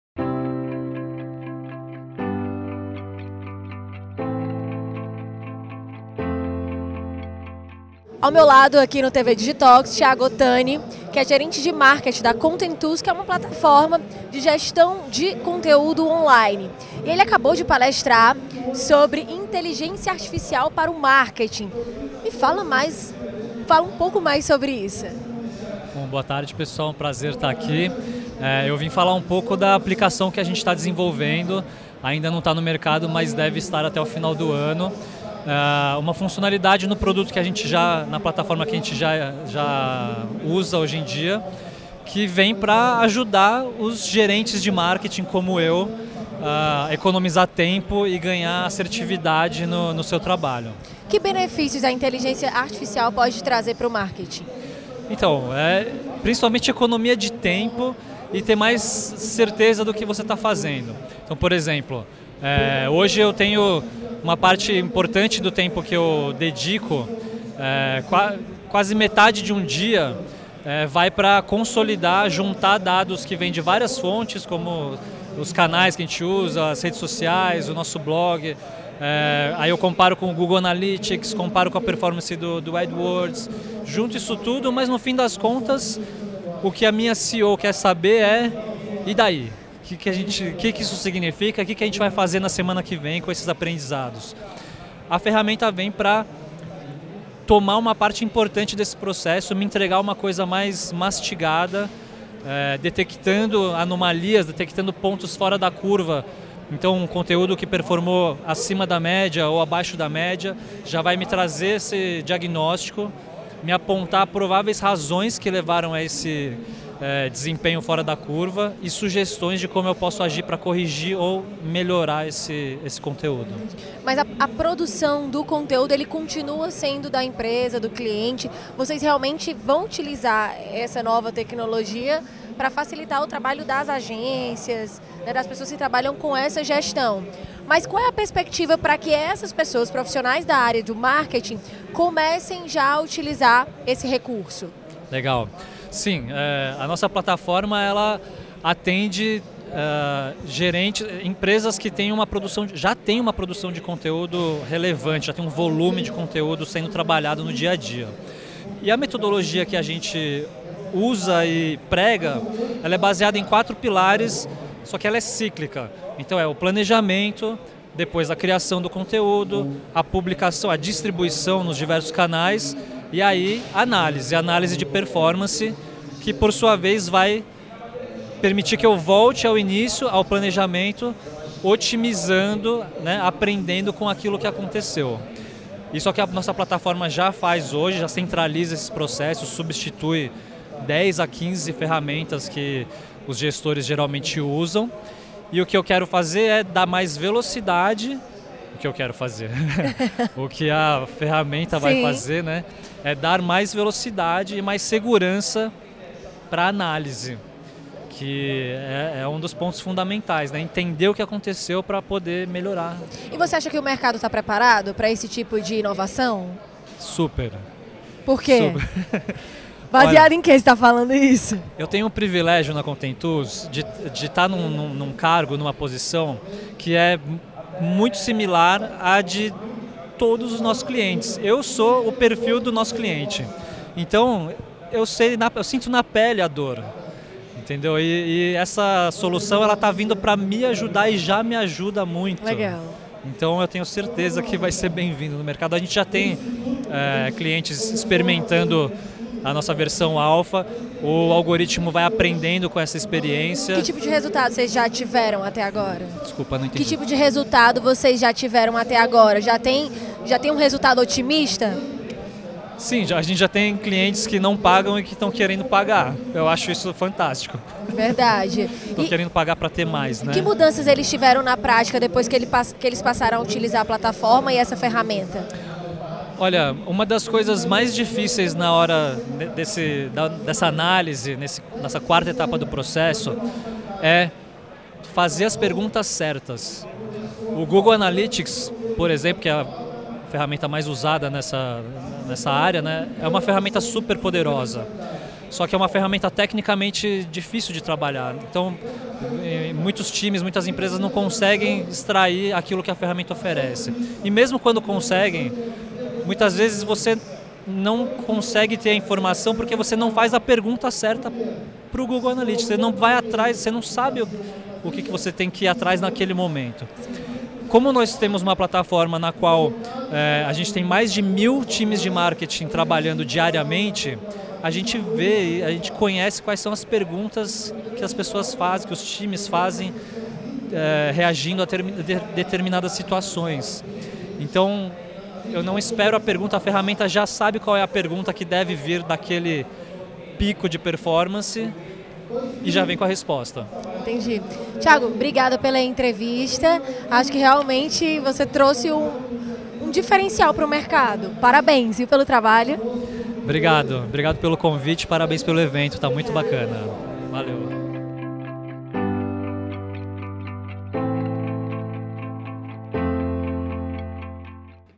Como a Inteligência Artificial pode aprimorar o marketing digital
Tiago Otani, Gerente de Marketing da Contentools, explica os principais benefícios e facilidades que a Inteligência Artificial traz para o Marketing. Assista a entrevista completa.

COMPARTILHE CONHECIMENTO. ;)